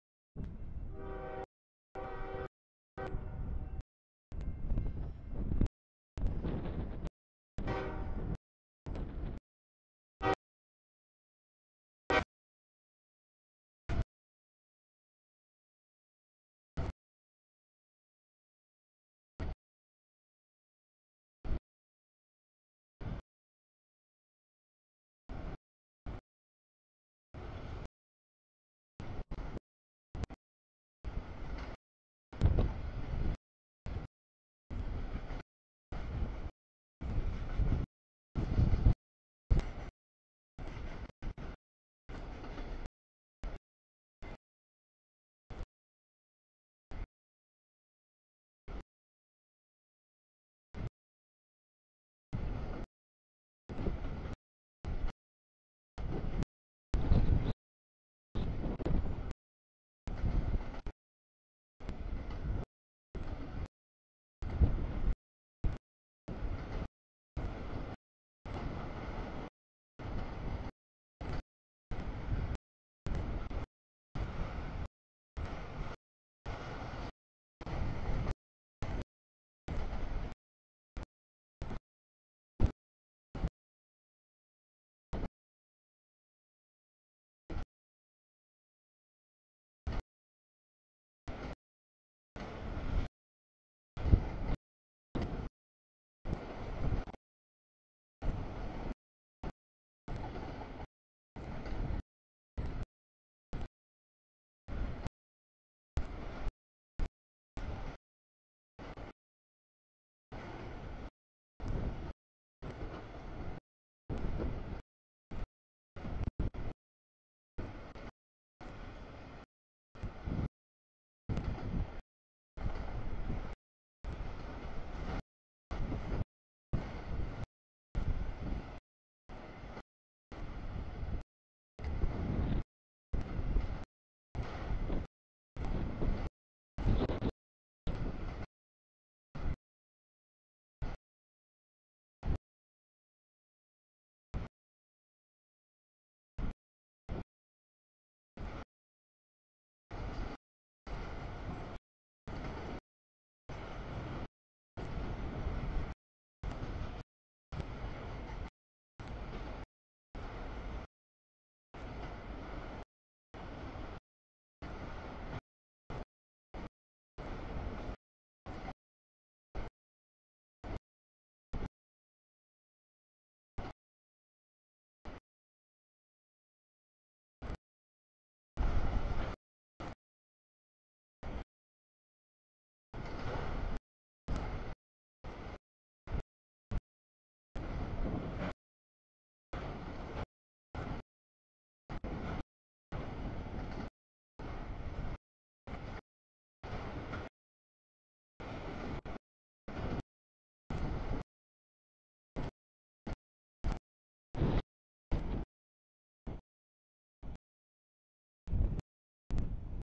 2- 25-2019 09:09 nb auto rack CSX 829, 4235 Q253
00:01 nb stack CSX 3315 Q157
00:19 nb stack trailers CSX 3373, 729 Q009e
00:28 nb stack CSX Q159
01:47 sb stack CSX Q160
02:30 nb auto rack CSX Q261
04:07 nb mix CSX 4792 Q438
07:33 nb stack trailers CSX 3474, 792 L003
08:57 sb stack CSX 745, 3174 Q158
09:09 nb auto rack CSX 829, 4235 Q253
09:54 sb CSX 3216, 3357 Q008
10:13 nb nysw cut, mix, racks CSX 3426 DPU 3415 Q410
11:45 nb mix CSX 3429, 3409, 6206 Q190
13:04 nb mix CSX 3385 with DPU 3381 Q439
13:49 sb stack trailers CSX 869, 7789 Q010